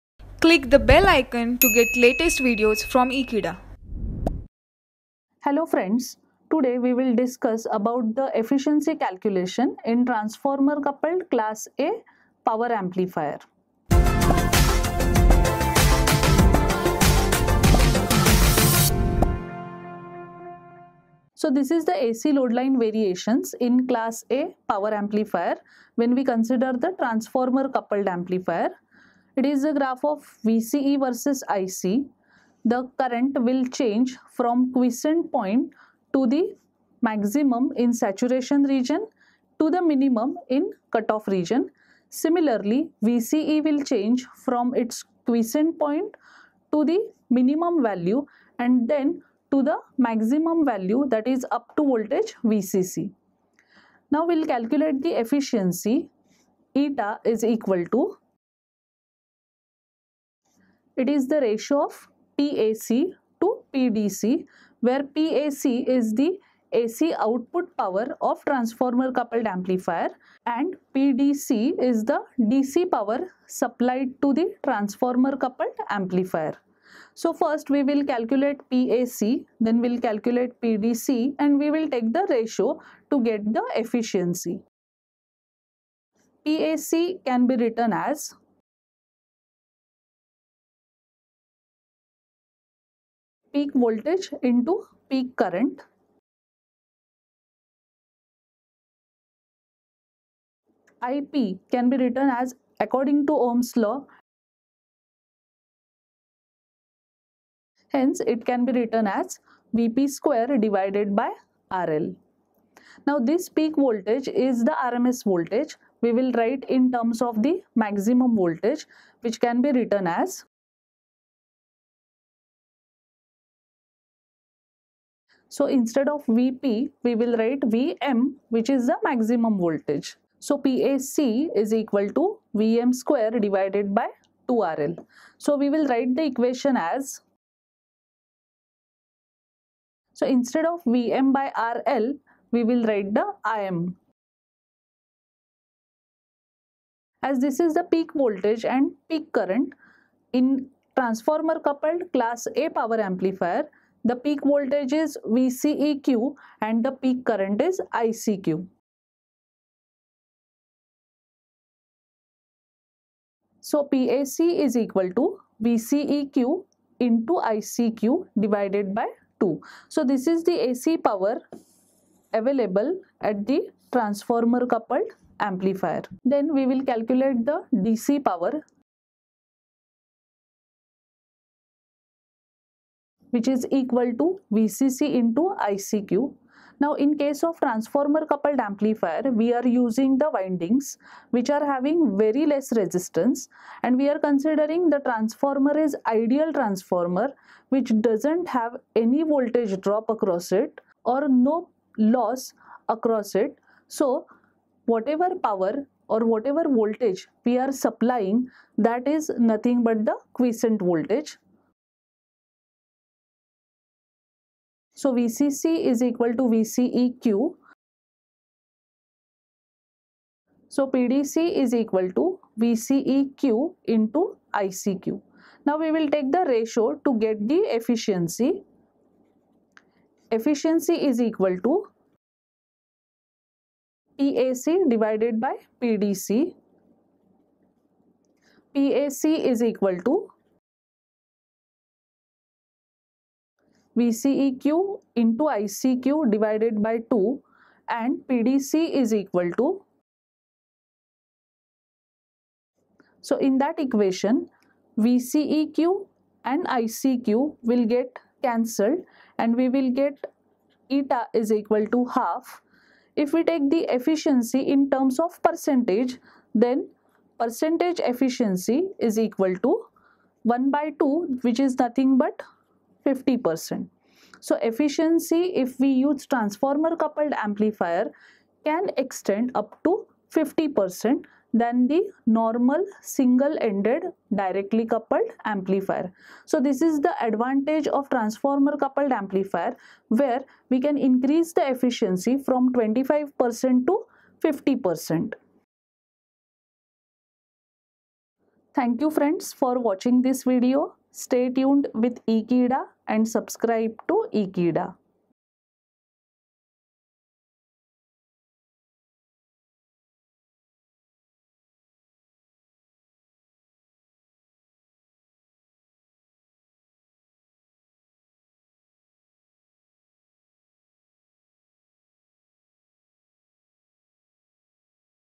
Efficiency Calculation in Transformer Coupled Class A Power Amplifier - Applied Electronics
Subject - Applied Electronics

Video Name - Efficiency Calculation in Transformer Coupled Class A Power Amplifier

Chapter - High Power Amplifiers

Faculty - Prof. Shilpa Gaikwad

Happy Learning!